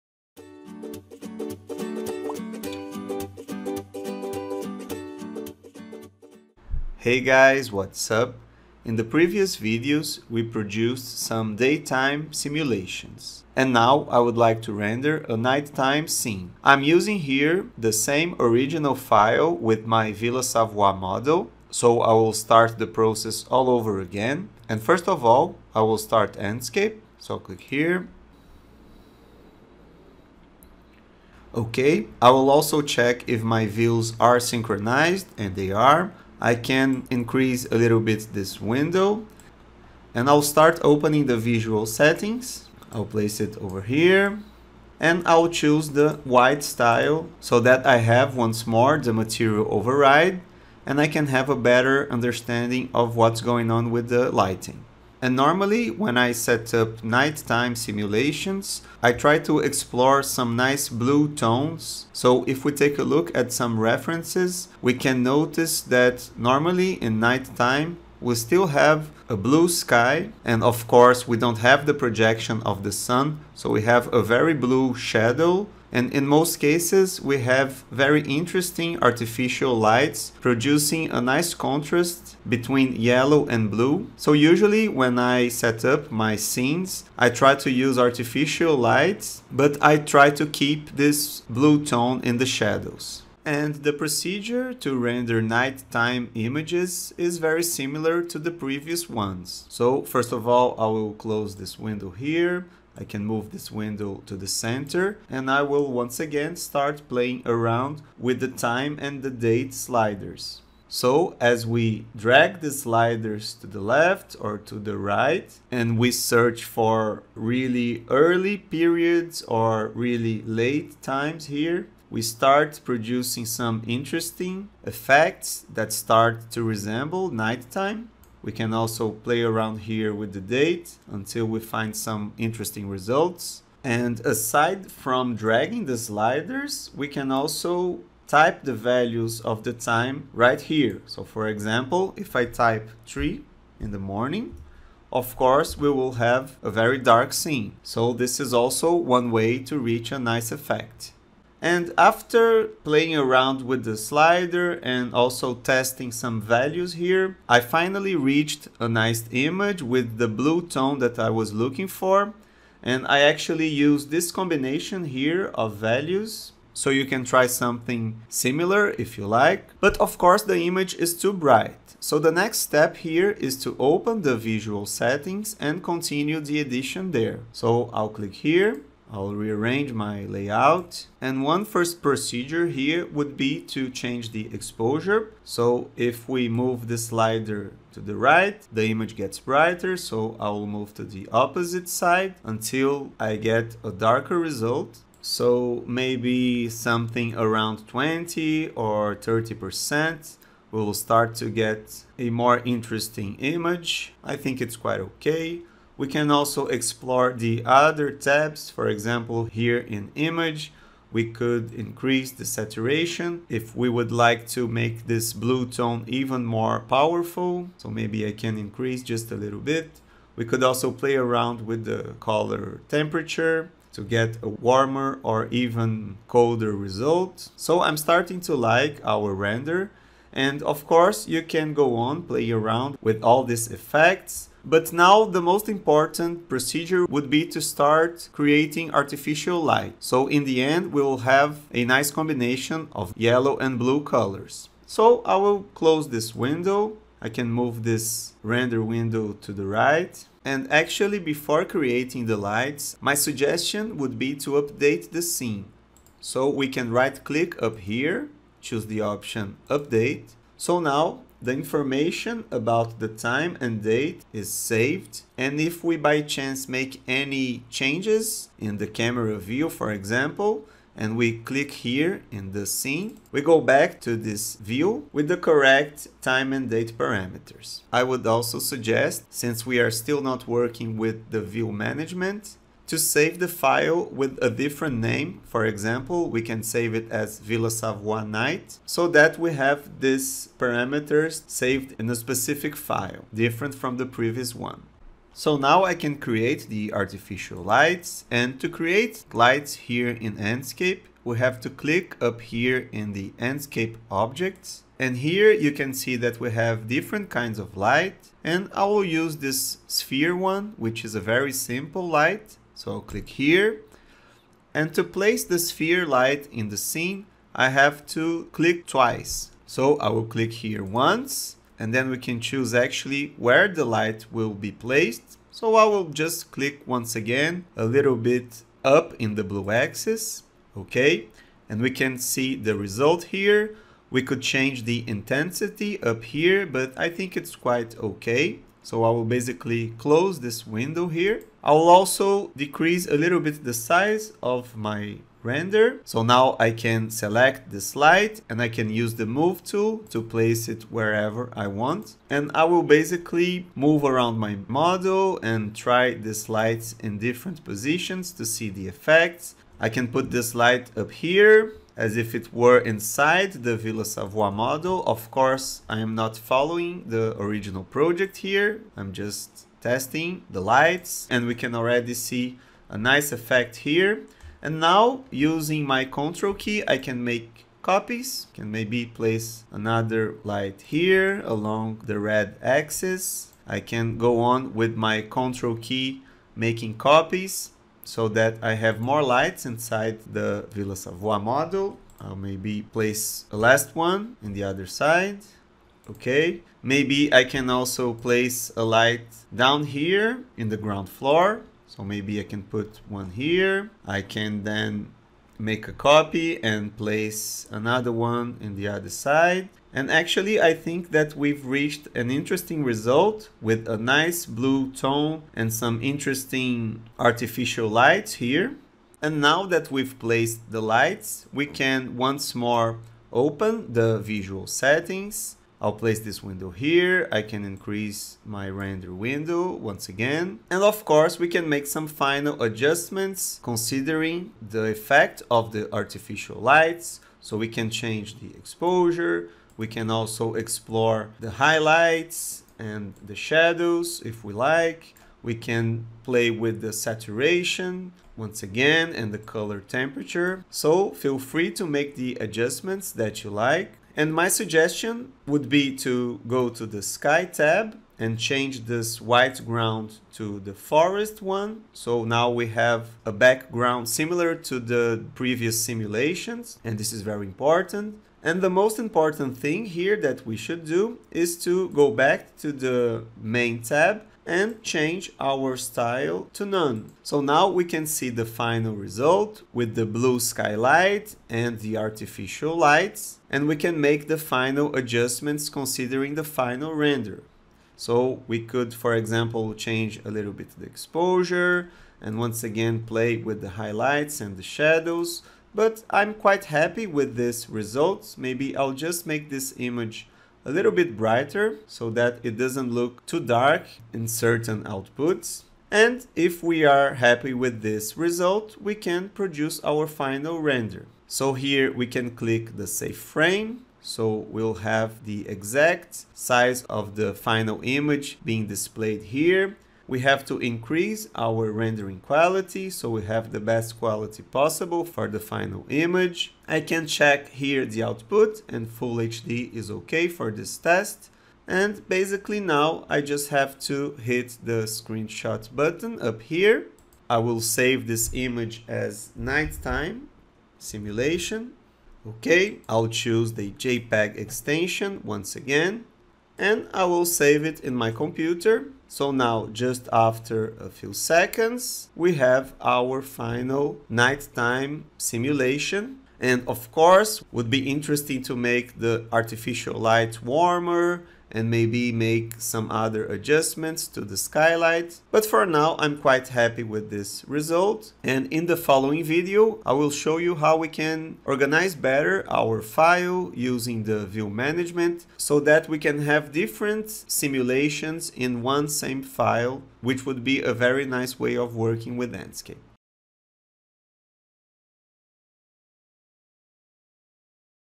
EXTERIOR NIGHTIME LIGHTING IN ENSCAPE
In this video I show you how I normally setup exterior nightime simulations in Enscape 3.4. I try to keep a blue skylight and combine it with artificial lights - and I play around with the time and date setup and afterwards I configue the visual settings.